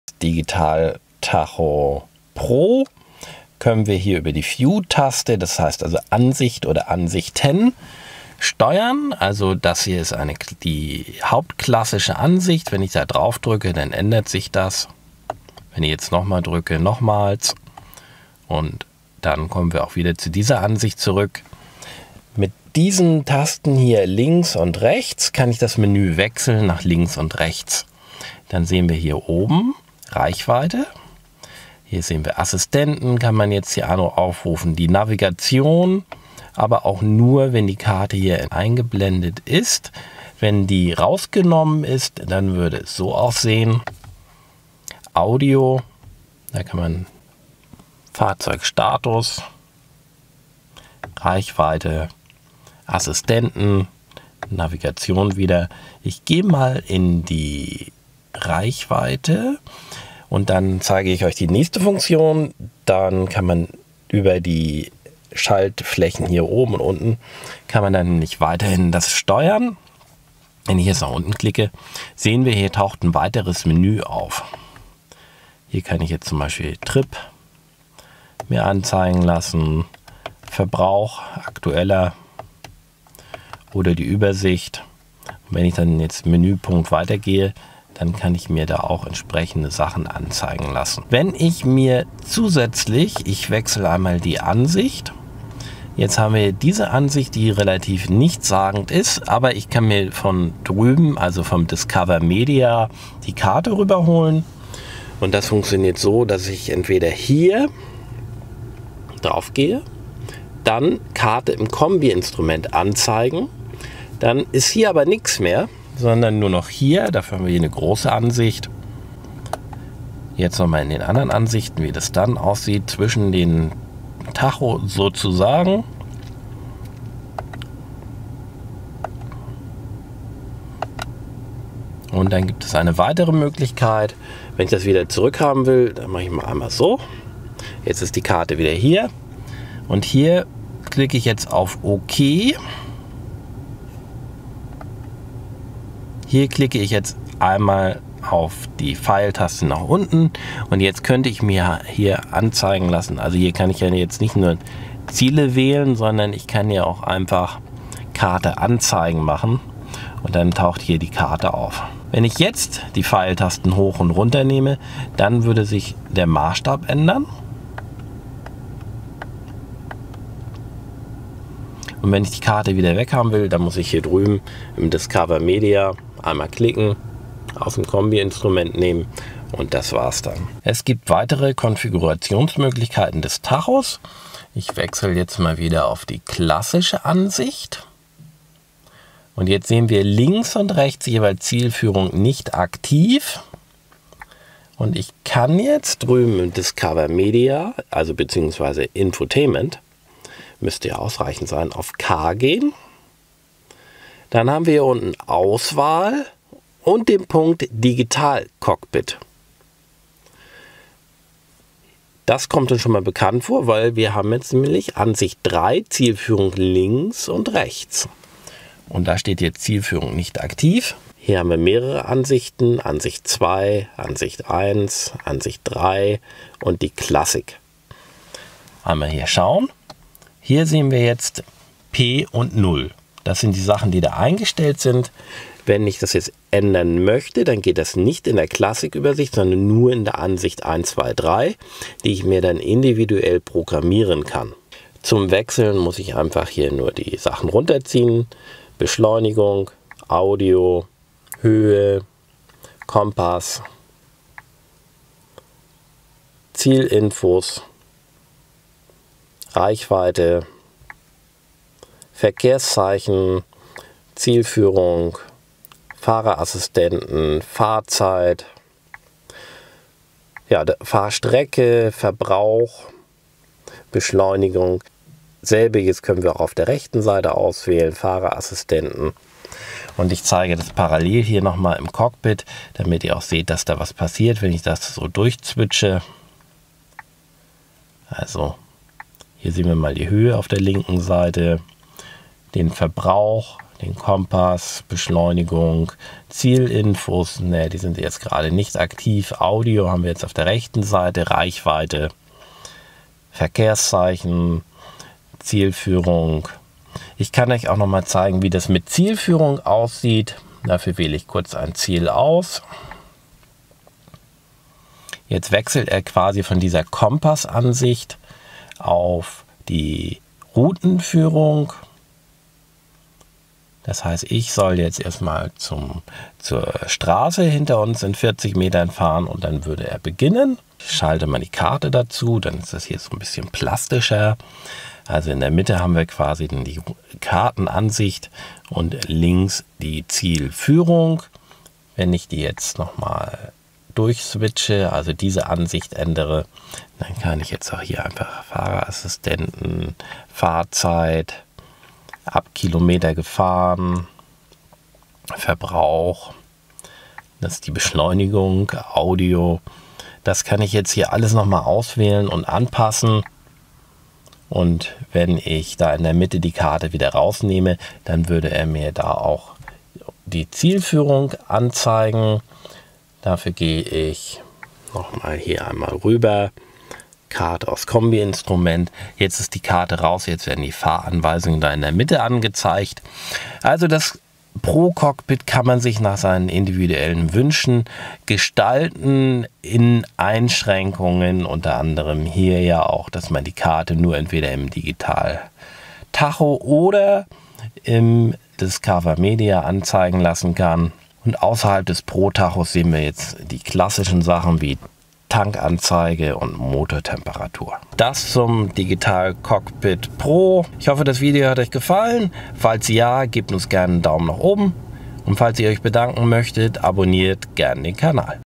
Digital Cockpit Pro im VW T-ROC 1.5 TSI OPF DSG 2024
Die Bedienung und Funktionsweise des Digital Cockpit Pro stellen wir im VW T-ROC 1.5 TSI OPF mit DSG des Modelljahres 2024 vor.